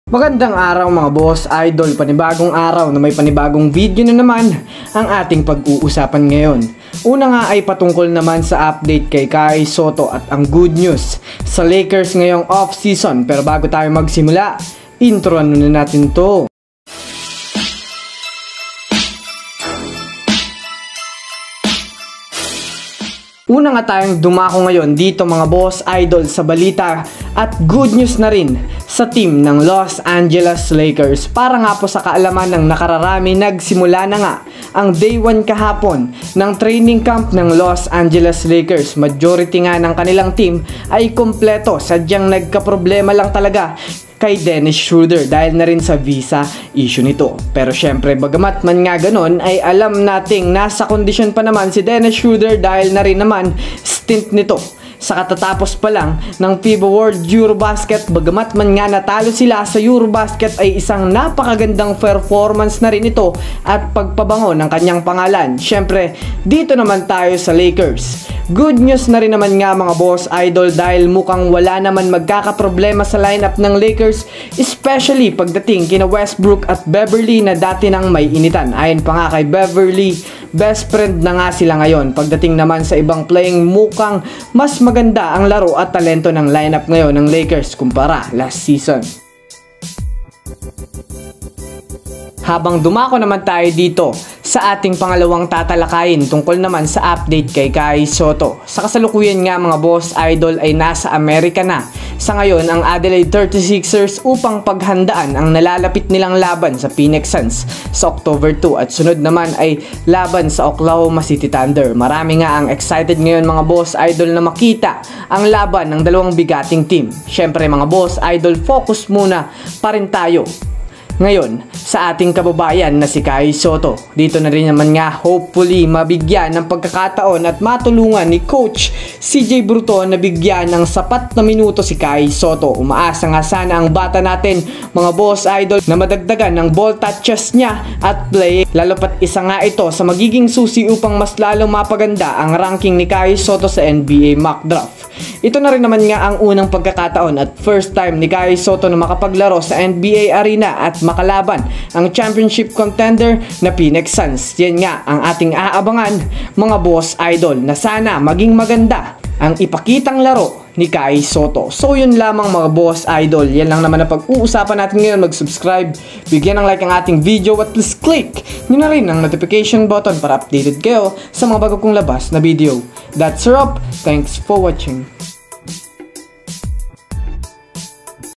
GOOD News kay KAI SOTTO vs Phoenix SUNS | LAKERS GAGAWA na ng PAGBABAGO
Halina't pag usapan natin ngayon ang balita kay Kai Sotto at Lakers.